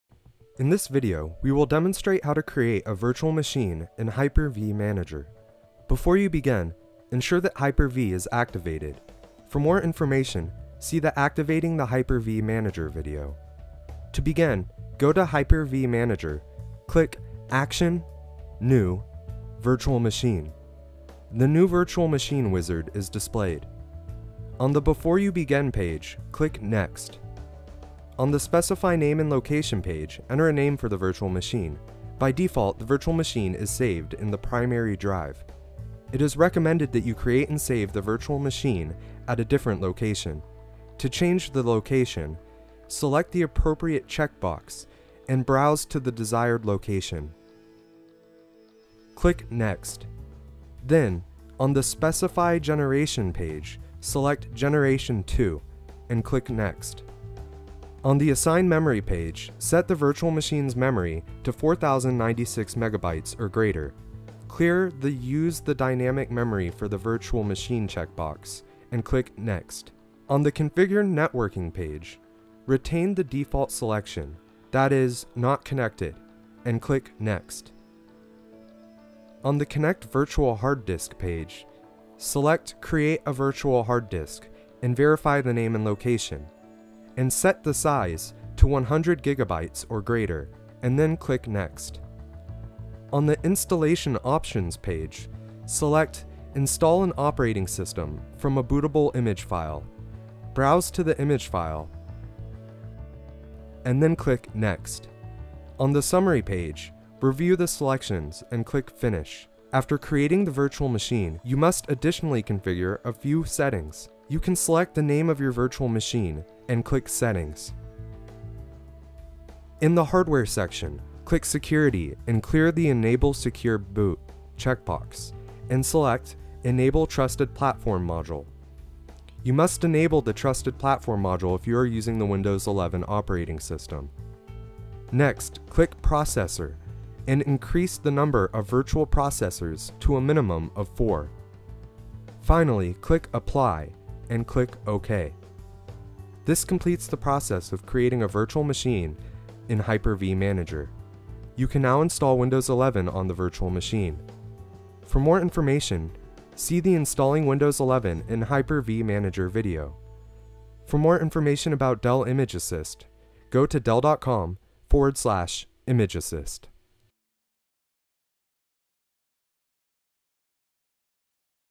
Create a Virtual Machine in Hyper-V Manager
This video demonstrates how to create a virtual machine in Hyper-V Manager.